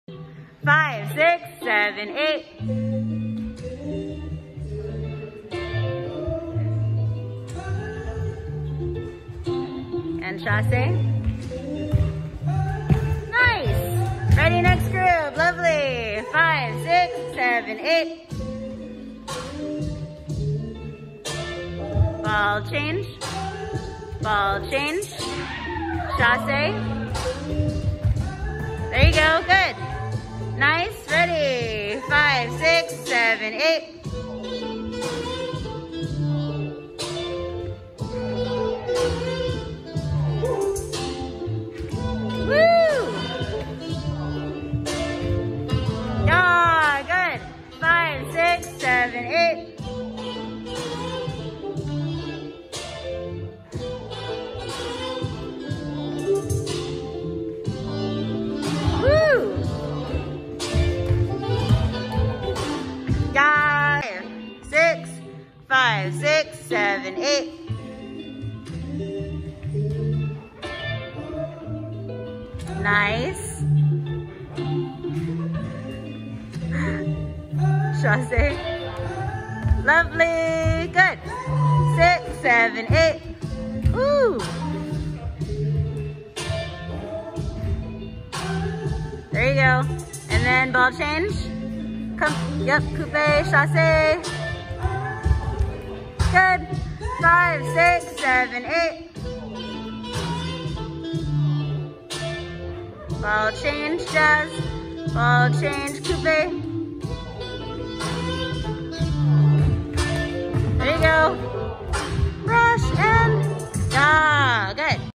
Pirouettes Jazz Progressions
Jazz Progressions: Turning 6 step, compass turn, ball change, outside jazz pirouette, ball change, coupé turn, chassé step grand jeté, step step developé jete.

Music copyright not intended.